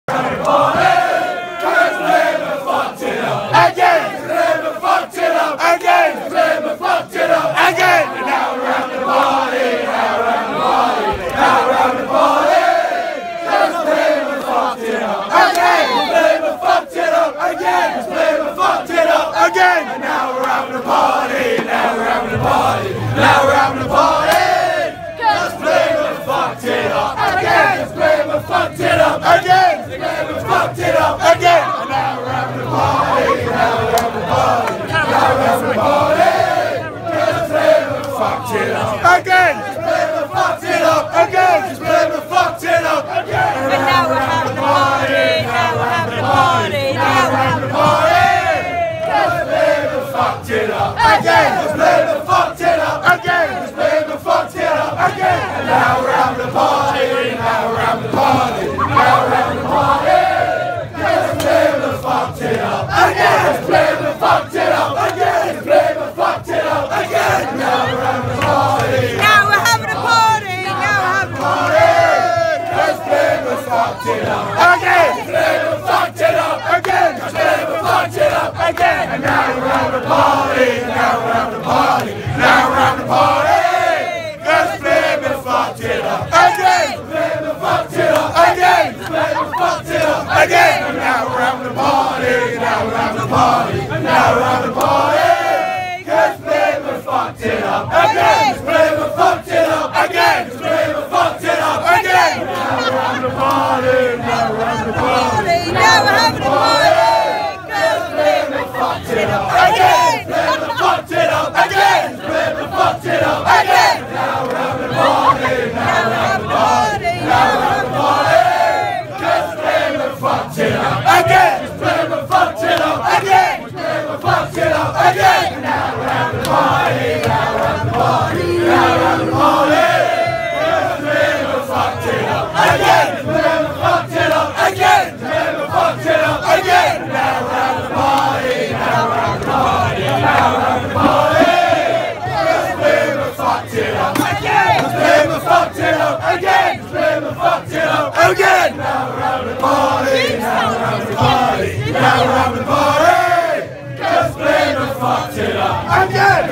Pompey fans in the Shepherds Crook | And now we're having a party.. 6/5/2017
Pompey fans singing in the Shepherds Crook after the match which sealed the League Two title on the final day